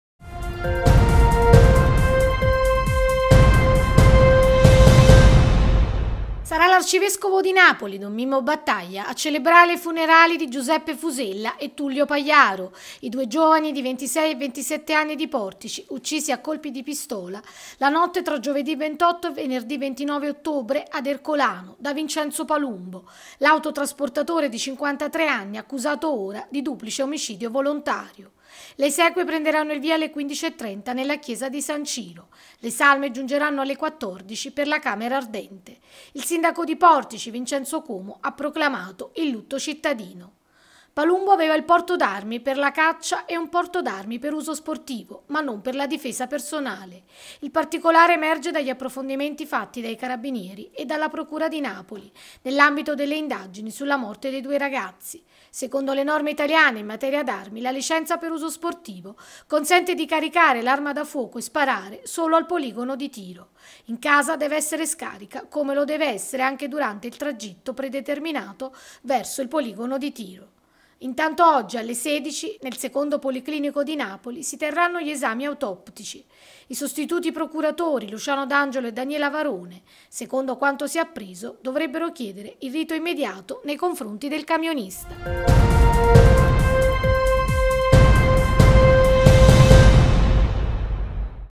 Studenti uccisi, domani i funerali a Portici. Celebra arcivescovo Napoli
Sara' l'arcivescovo di Napoli, don Mimmo Battaglia, a celebrare i funerali di Giuseppe Fusella e Tullio Pagliaro, i due giovani di 26 e 27 anni, di Portici, uccisi a colpi di pistola la notte tra giovedi' 28 e venerdi' 29 ottobre, ad Ercolano, da Vincenzo Palumbo, l'autotrasportatore di 53 anni accusato ora di duplice omicidio volontario. Le esequie prenderanno il via alle 15,30 nella Chiesa di San Ciro. Le salme giungeranno alle 14, per la camera ardente. Il sindaco di Portici, Vincenzo Cuomo, ha proclamato il lutto cittadino.
Palumbo aveva il porto d'armi per la caccia e un porto d'armi per uso sportivo ma non per la difesa personale. Il particolare emerge dagli approfondimenti fatti dai carabinieri e dalla Procura di Napoli nell'ambito delle indagini sulla morte dei due ragazzi. Secondo le norme italiane in materia d'armi, la licenza per uso sportivo consente di caricare l'arma da fuoco e sparare solo al poligono di tiro. In casa deve essere scarica come lo deve essere anche durante il tragitto predeterminato verso il poligono di tiro.
Intanto oggi, alle 16, nel Secondo Policlinico di Napoli si terranno gli esami autoptici: i sostituti procuratori di Napoli Luciano d'Angelo e Daniela Varone, secondo quanto si e' appreso, dovrebbero chiedere il rito immediato nei confronti del camionista.